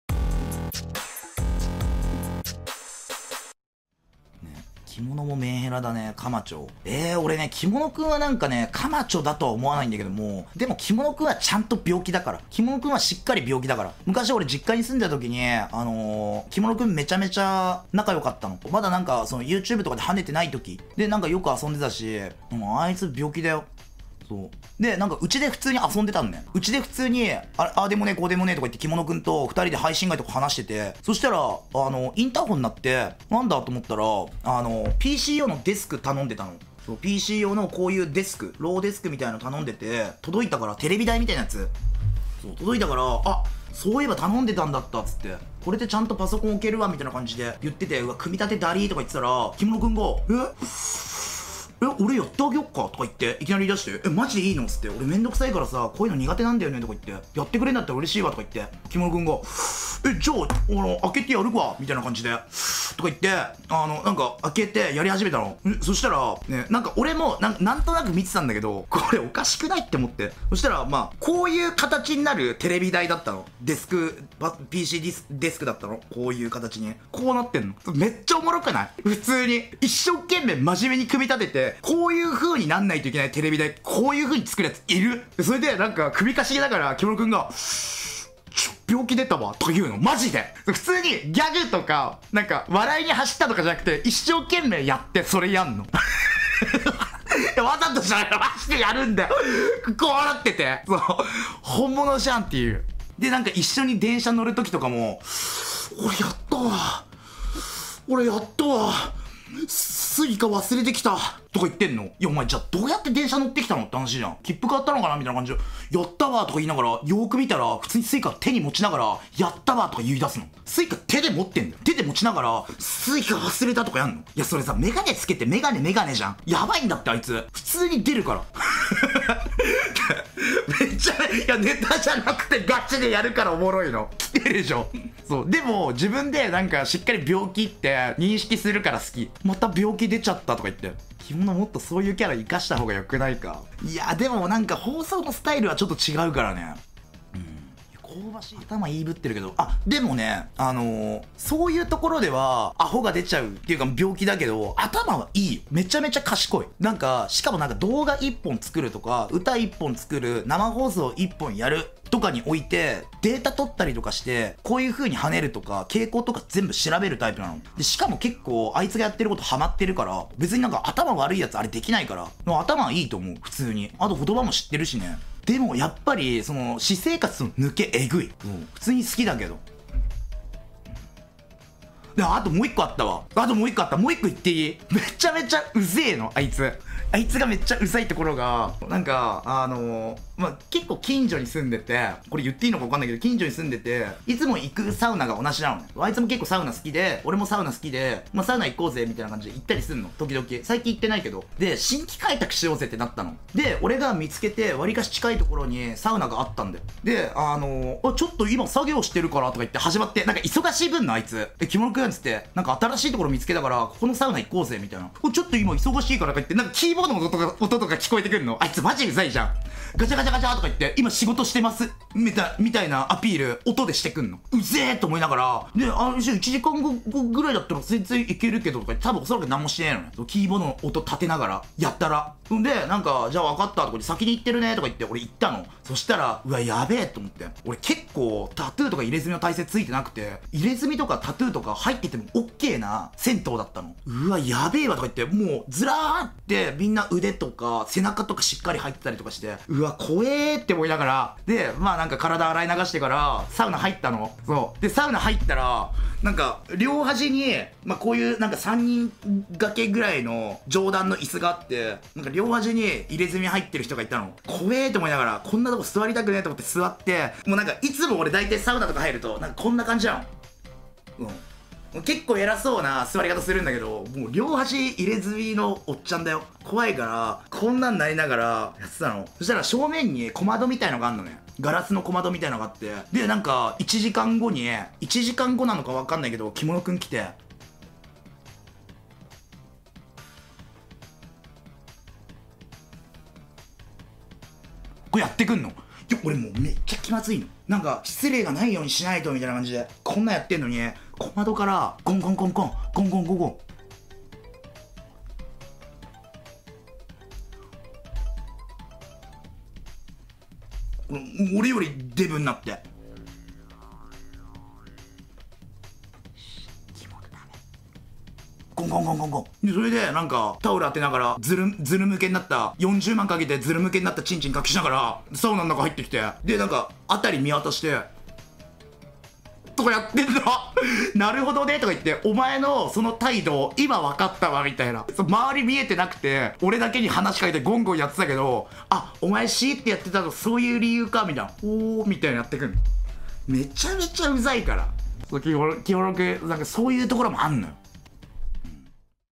[解説]kimonoちゃんは普段「病気の人」。ただこの人頭がめちゃキレる[なあぼう/kimonoちゃん/切り抜き/生放送/友達/サウナ/刺青/爆笑/クリエイティブ/配信者/ツイキャス]
0:00 Kimonoちゃんは「病気だよ」
0:23 Kimonoちゃんが病気が出る瞬間
2:27 普段は抜け多いが頭はかなりキレる
3:00 サウナで「病気」が出た瞬間

なあぼう(ツイキャス配信者)の雑談生放送の見どころや
役立つ情報、論破を切り抜き動画として投稿しています。